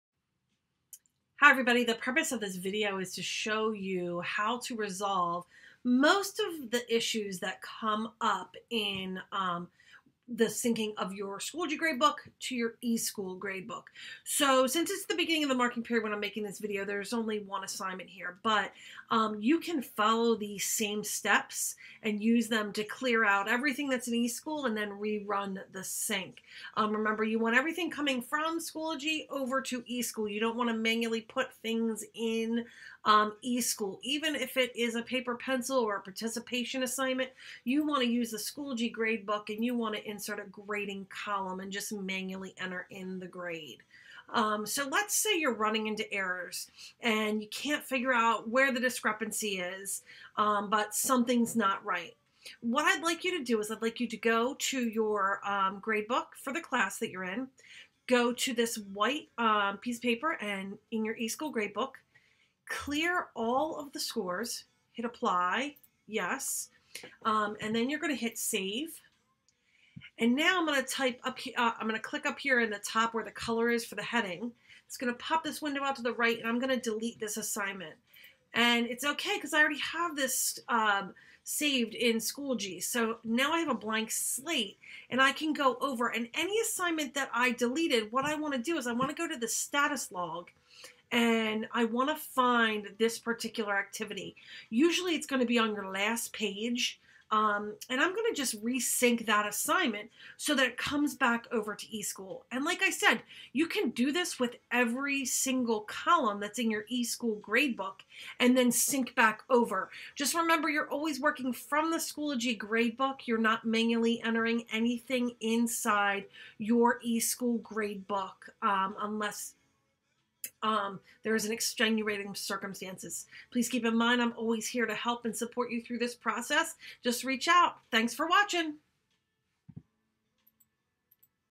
Solve Issues with eSchool Schoology Gradebook Sync
This video shows educators how to resolve virtually issues with eSchool Schoology gradebook.  After you have checked other common troubleshooting issues, this video walk you through how to resolve issues by clearing scores in eSchool and then rerunning the sync.